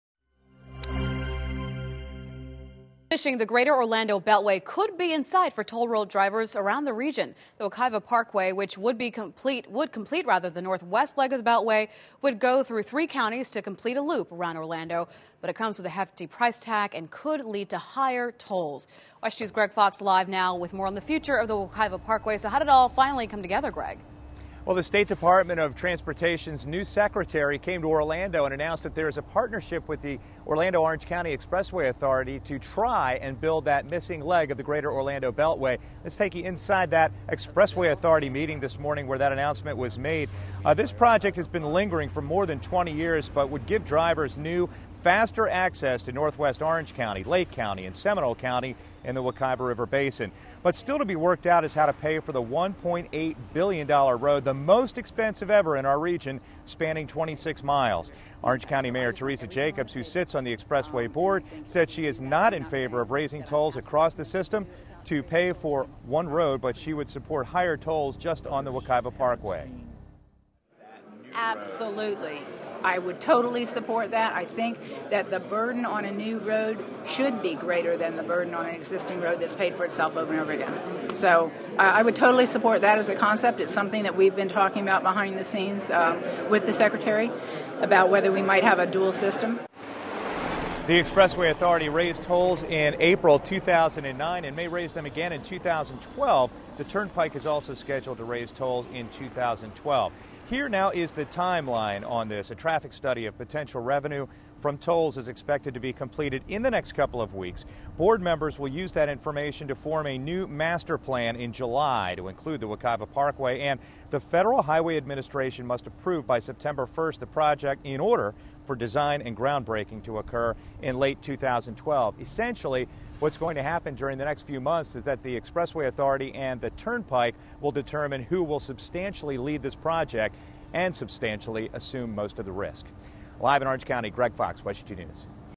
Will Parkway Lead To Toll Hikes?
The Wekiva Parkway may get a green light soon.